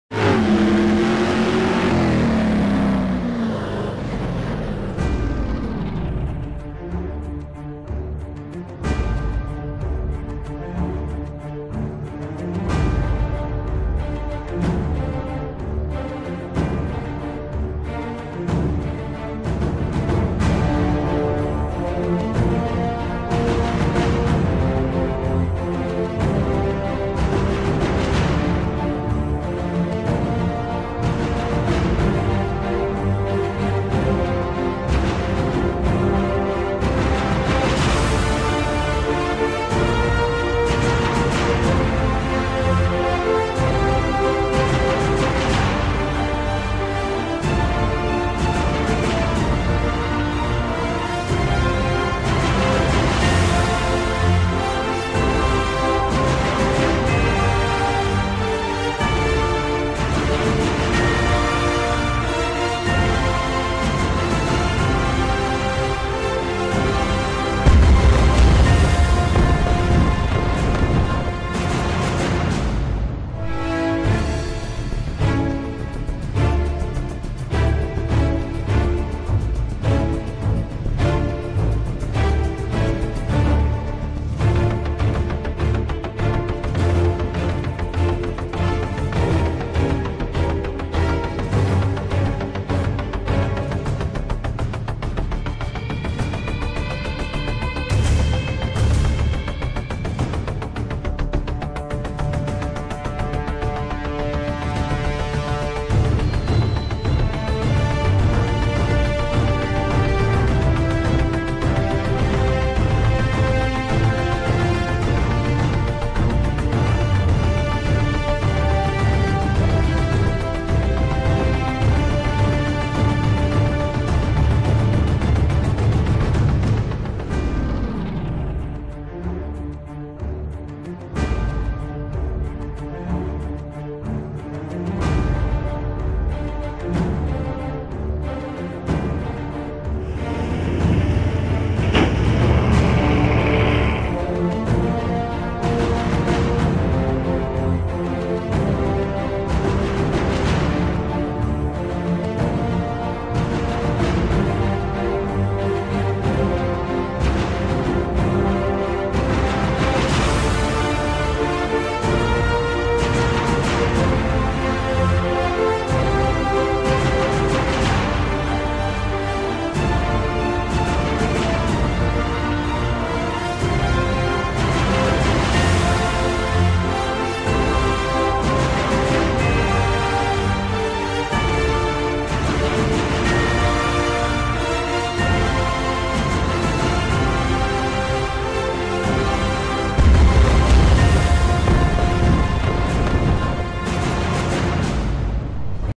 Ghe-O Rescue - Der Offroad-Retter
Noch mehr Monstertrucks - Klick Hier!

Sieht aus, wie die Strafe Gottes und ist größer als ein Hummer H1. Dabei will er doch bloß helfen! Der Geh-O Rescue kommt aus Rumänien und soll dort im unwegbaren Gelände Menschen retten.